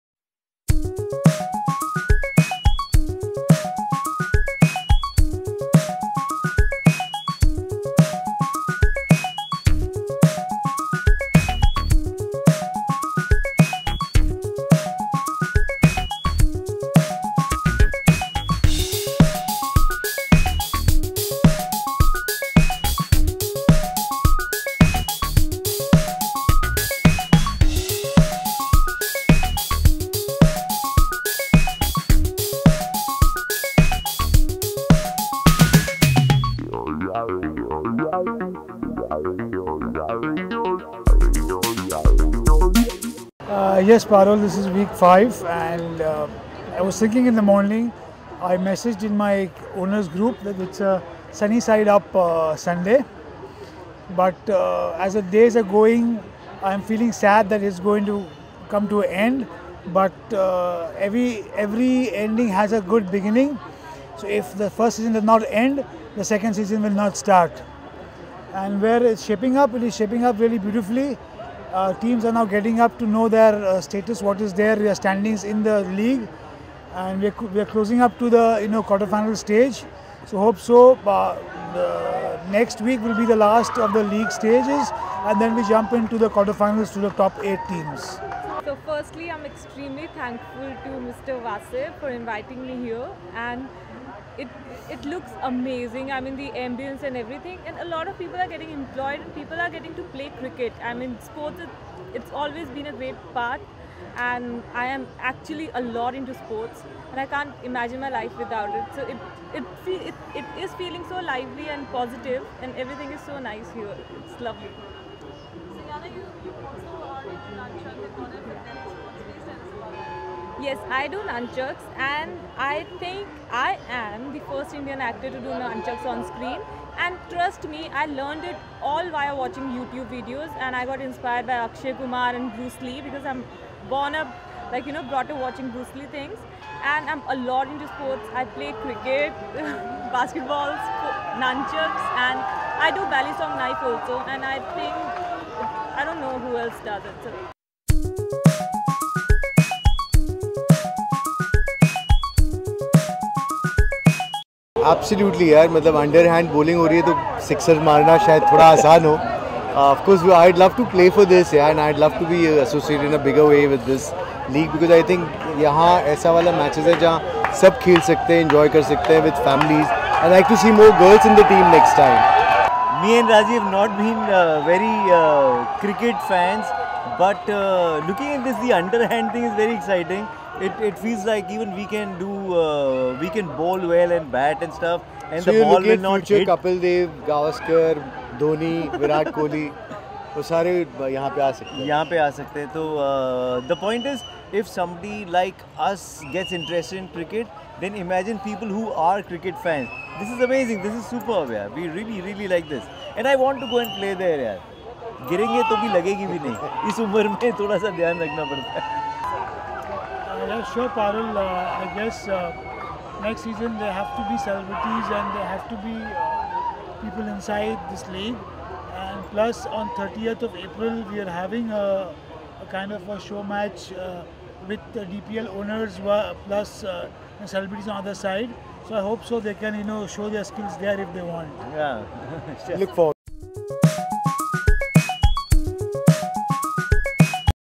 Dreams Premier League - DPL 2019 - Day 9 - Wasib Peshimam
Week 5  Day 9 Dreamz Premier League Spearheaded by Wasib Peshimam witnesses Bollywood and Television Actor Eijaz Khan, Dynamic Politician Mr.Babloo Aziz, Biker Author and Entrepreneur Mr.Kapil Pathare.